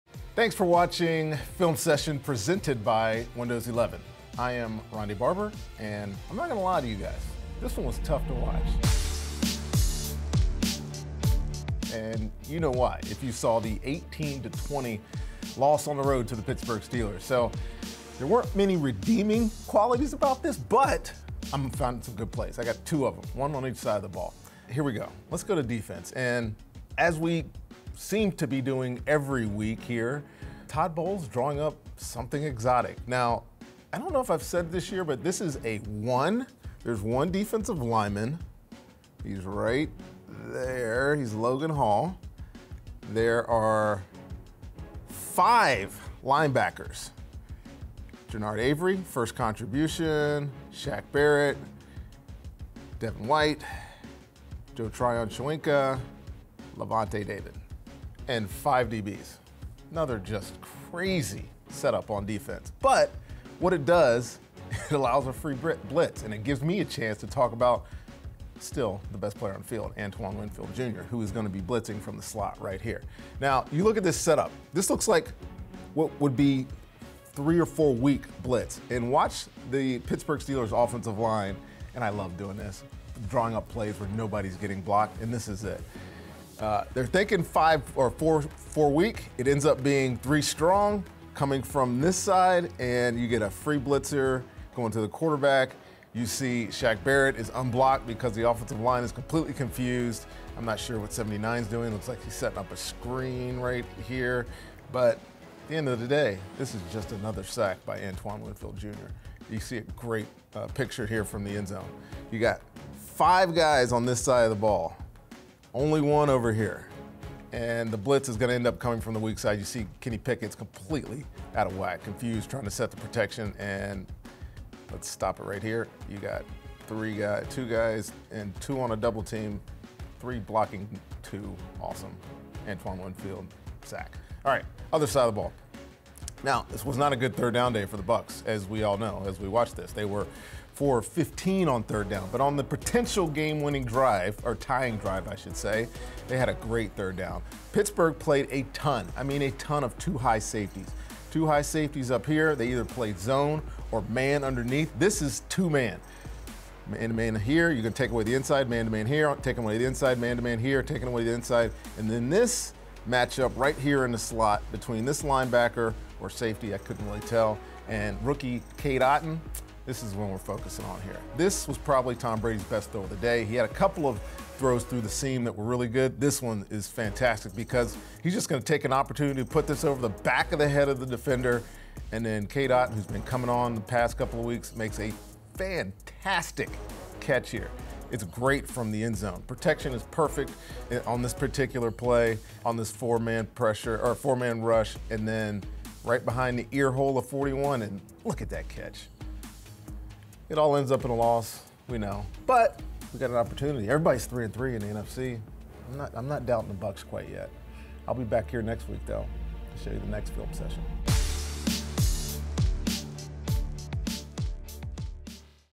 Ronde Barber Breaks Down Key Plays From Week 6 vs. Pittsburgh Steelers | Film Session
Legendary DB Ronde Barber breaks down some of the best plays from the Week 6 loss to the Pittsburgh Steelers.